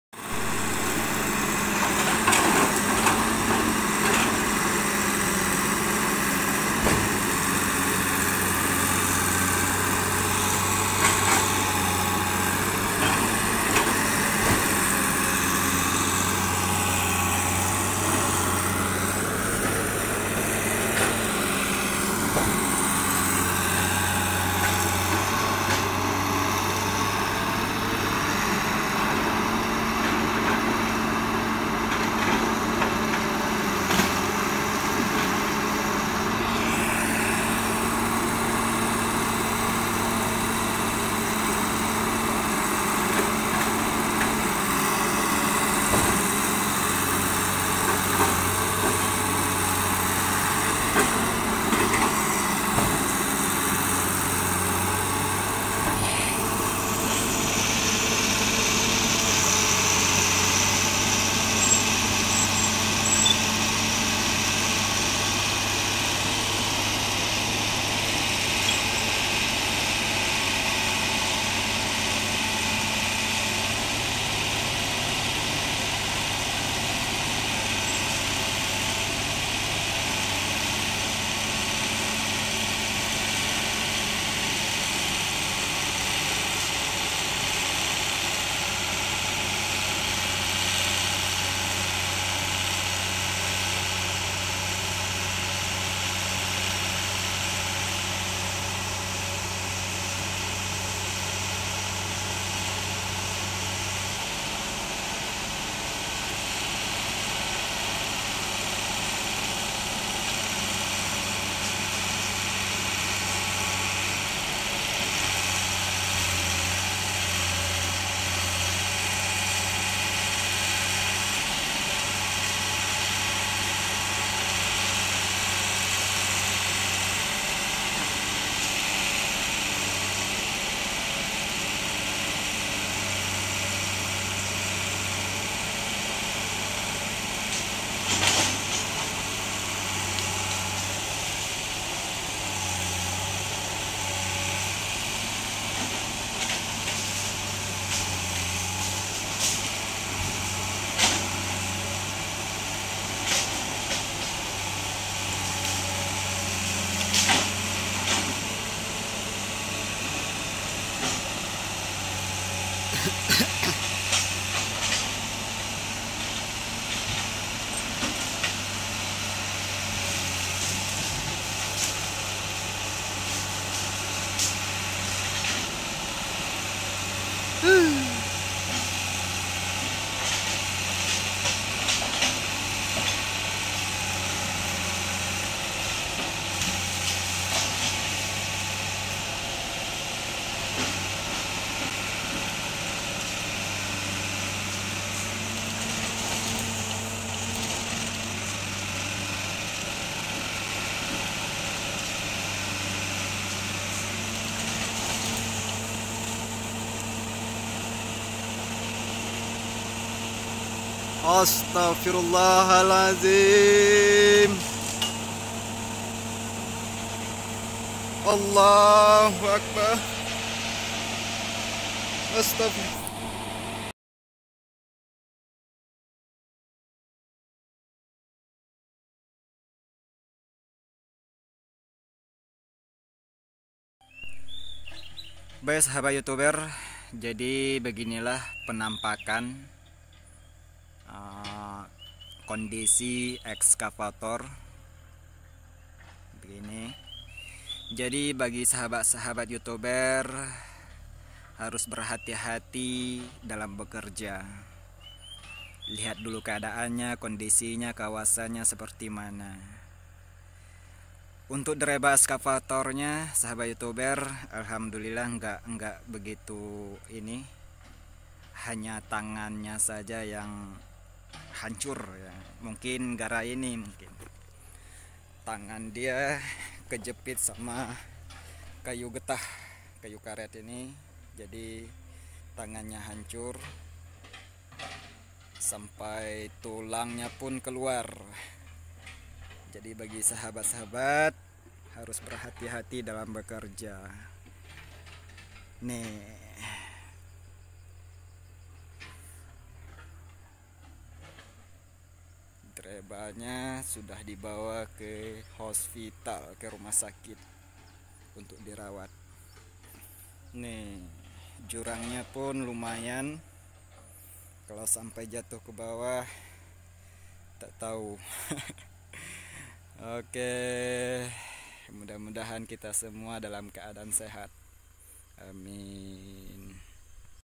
Part 2→27 April 2022 detik detik excavator excident nyaris masuk jurang
channel@lubdayat3882 video ini tentang excavator nyaris masuk jurang,Dreba yang pemberani menantang maut mengakibatkan balapetaka,musibah bulan romadhan sebelum raya
excavatorHeavy Equipment Accident, accident news, accident today, accident compilation, excavator accident, accident at work, excuvator fails
#27 April 2022 accident